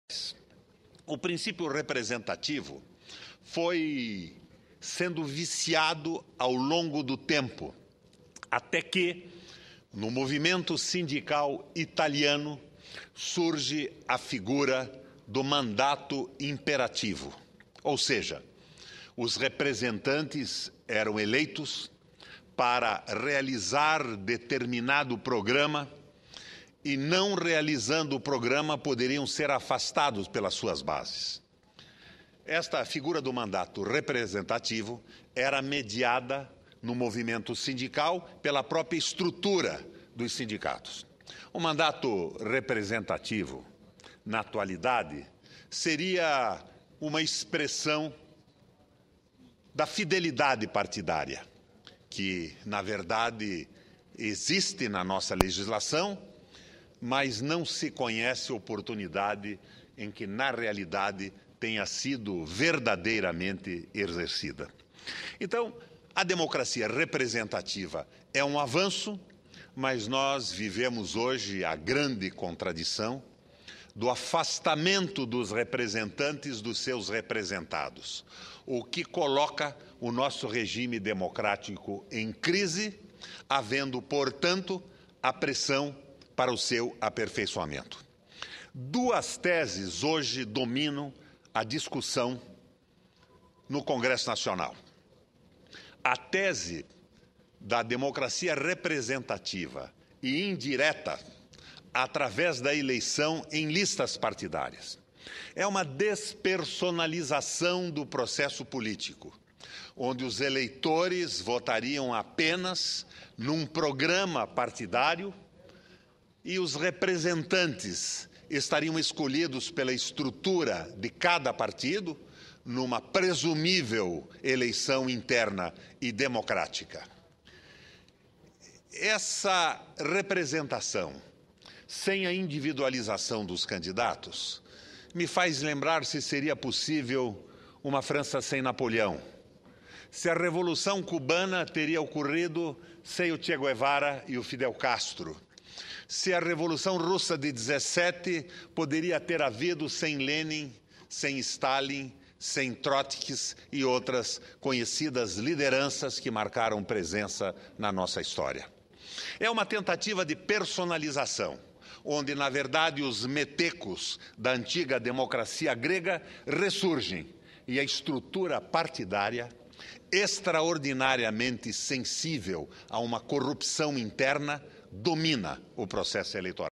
Sen. Roberto Requião fala do desvirtuamento do princípio do mandato representativo
28/03/2011 - Plenário - Sen. Roberto Requião fala do desvirtuamento do princípio do mandato representativo